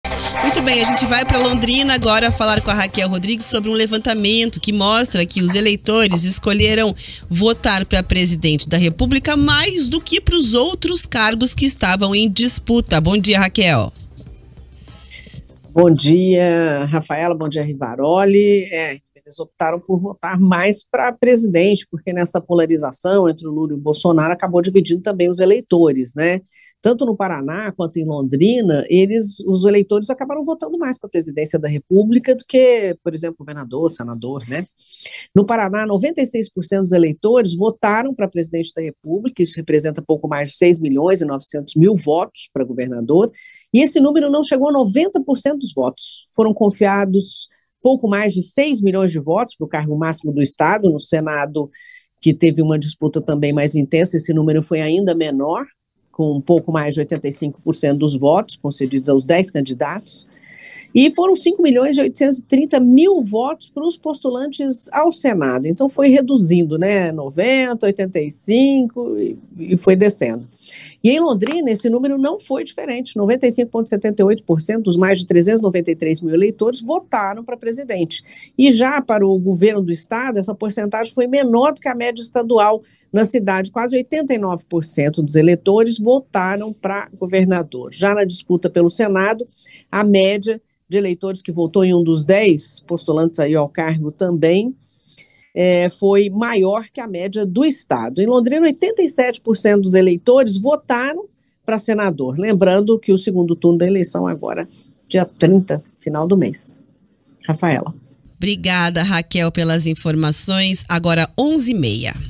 Eleições 2022: população londrinense votou mais em Presidente do que nos demais cargos
Um levantamento mostra que os eleitores de Londrina optaram por votar para Presidente da República mais do que para os outros cargos que estavam em disputa.